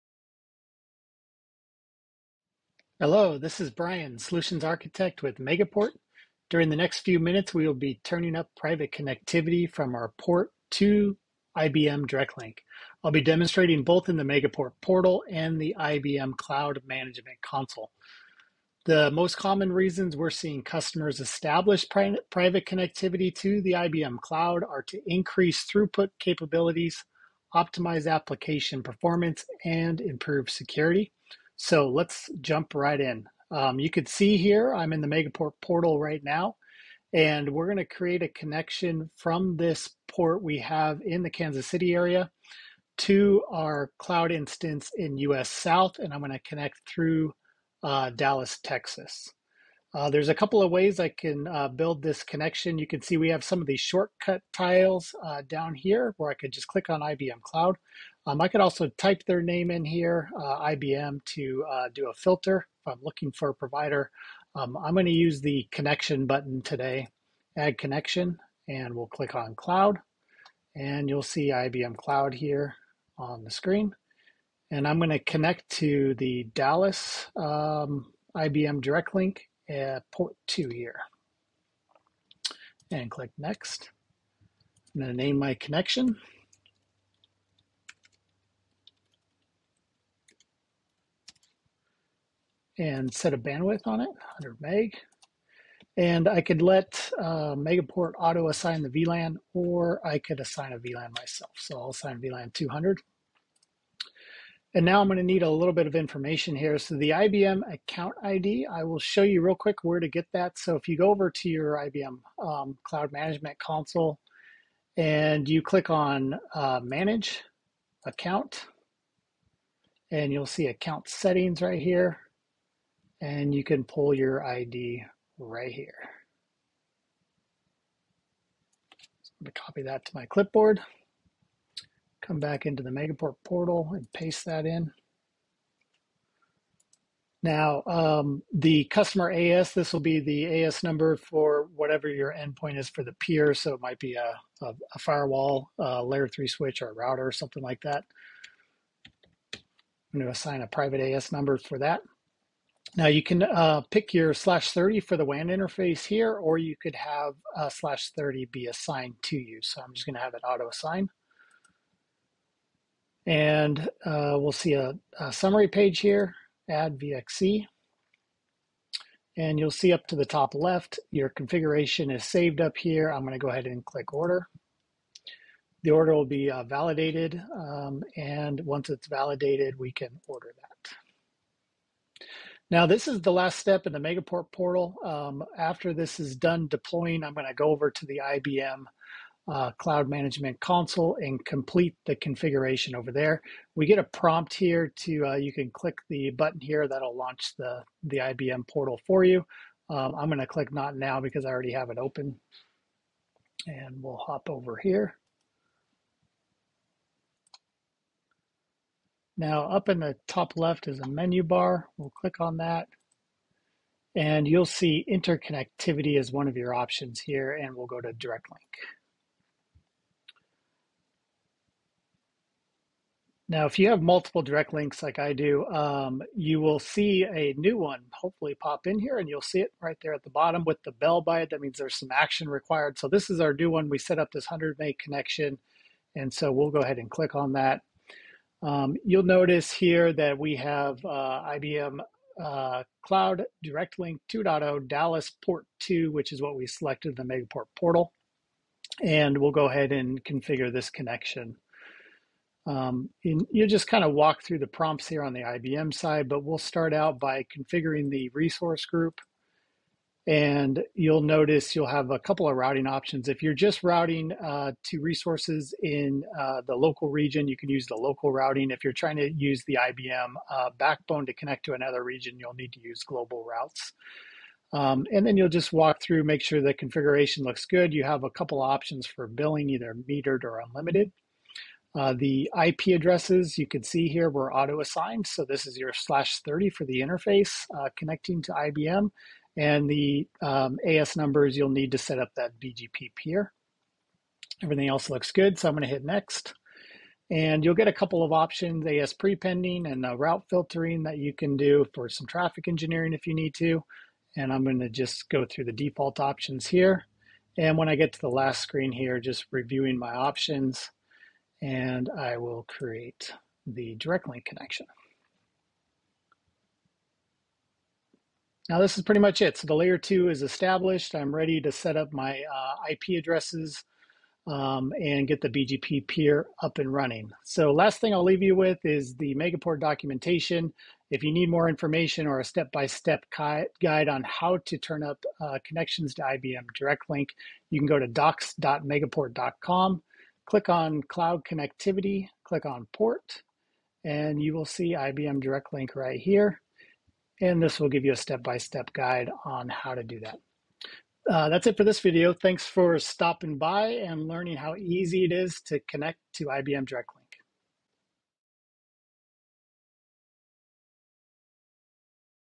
Private Connectivity to IBM Cloud - Portal Demo
Brian Bowman, Solutions Architect at Megaport, will guide you through the process of setting up private connectivity to IBM Cloud via IBM Direct Link, using both the Megaport portal and the IBM Cloud Management Console.

This video is perfect for customers who want to enhance their throughput capabilities and establish a secure connection to the IBM Cloud. Get ready to learn and take action!

Have a question, or would like someone from Megaport to follow up with you? Please complete this form, and we'll speak to you shortly